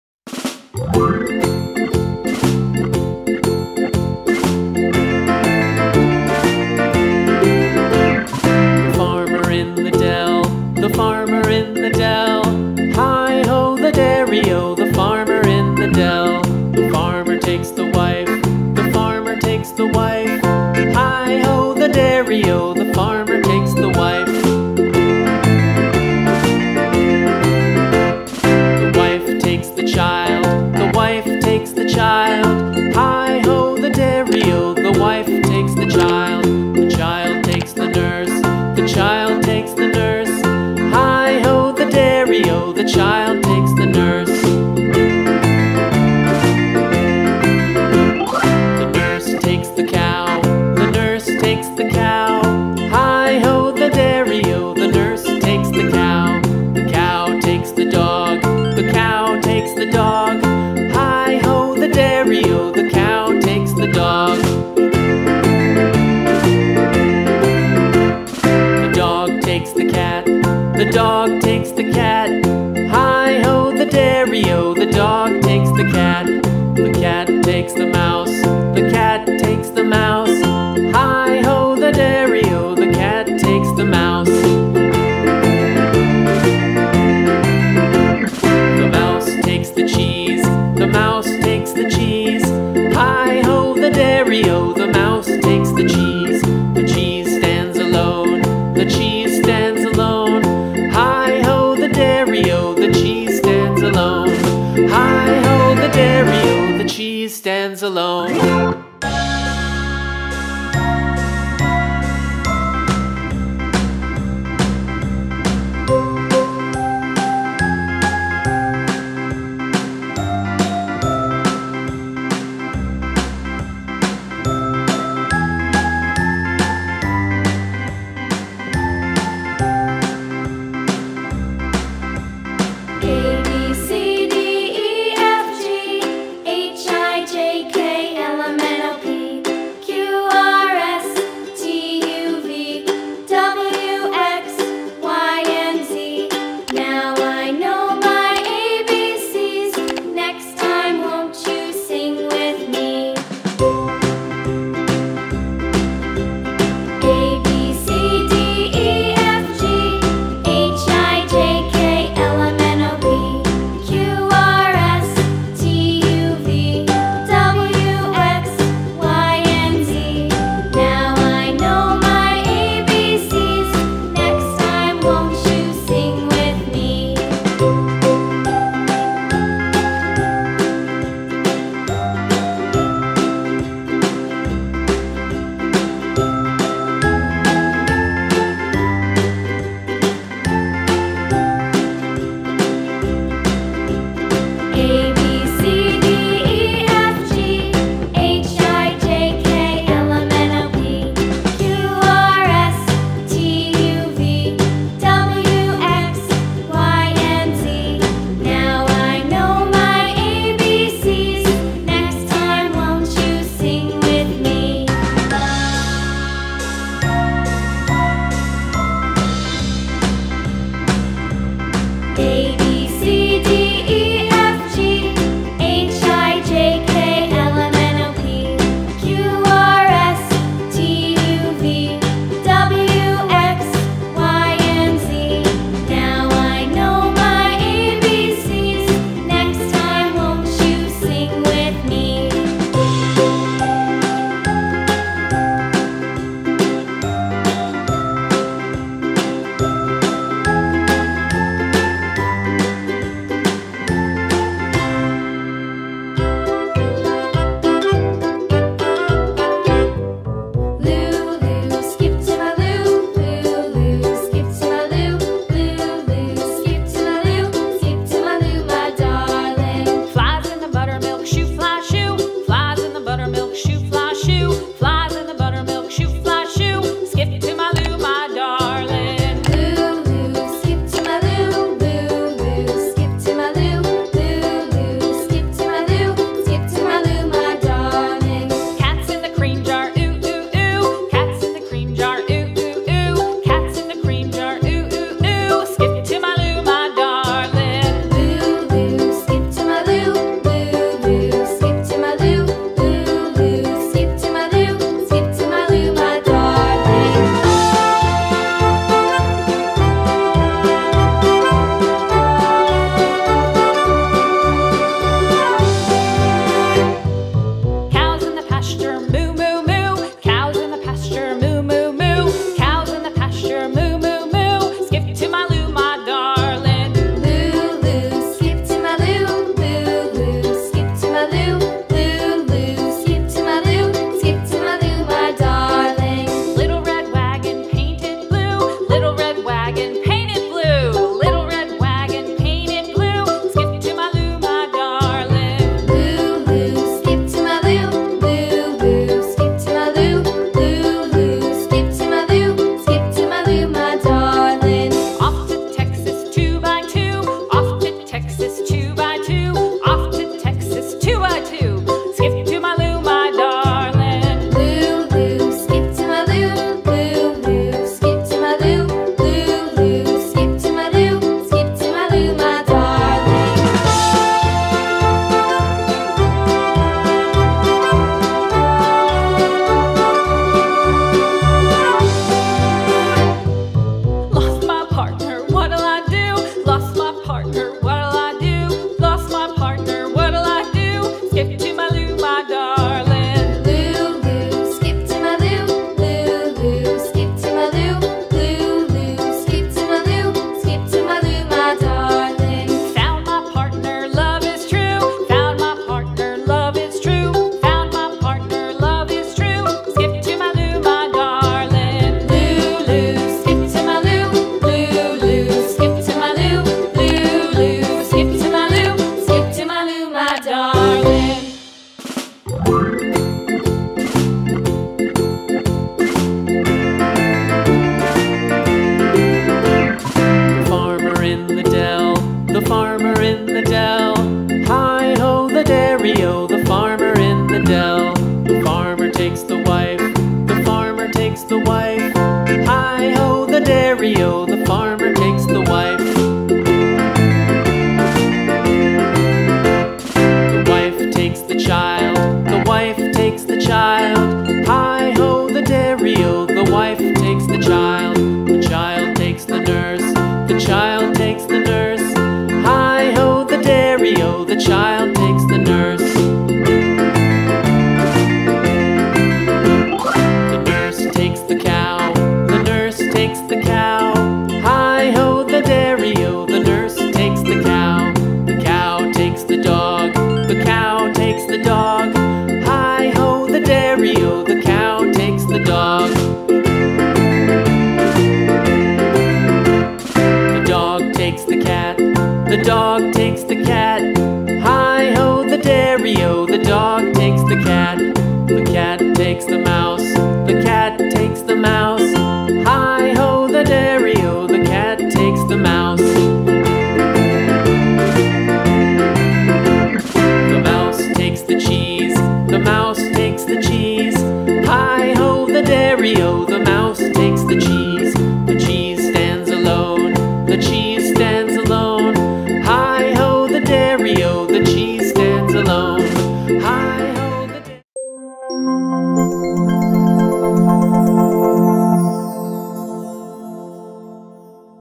HOW TO DRAW A HOUSE EASY STEP BY STEP I House Drawing, Painting, Coloring for Kids
Let's learn how to draw a house easy drawings together. Come and paint with me...